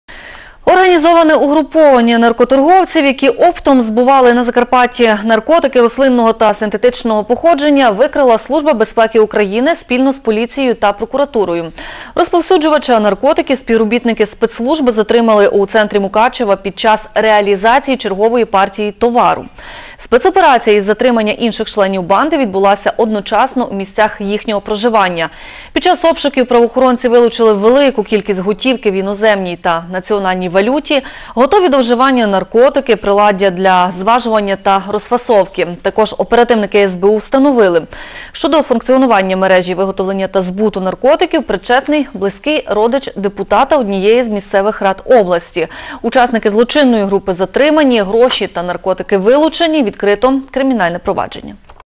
У Мукачеві працівники СБУ затримали наркоторговця
Організоване угруповання наркоторговців, які оптом збували на Закарпатті наркотики рослинного та синтетичного походження викрила Служба безпеки України спільно з поліцією та прокуратурою. Розповсюджувача наркотиків співробітники спецслужби затримали у центрі Мукачева під час реалізації чергової партії «товару». Спецоперація із затримання інших членів банди відбулася одночасно у місцях їхнього проживання. Під час обшуків правоохоронці вилучили велику кількість готівки в іноземній та національній валюті, готові до вживання наркотики, приладдя для зважування та розфасовки. Також оперативники СБУ встановили, що до функціонування мережі виготовлення та збуту наркотиків причетний близький родич депутата однієї з місцевих рад області. Учасники злочинної групи затримані. Гроші та наркотики – вилучені. Відкрито кримінальне провадження.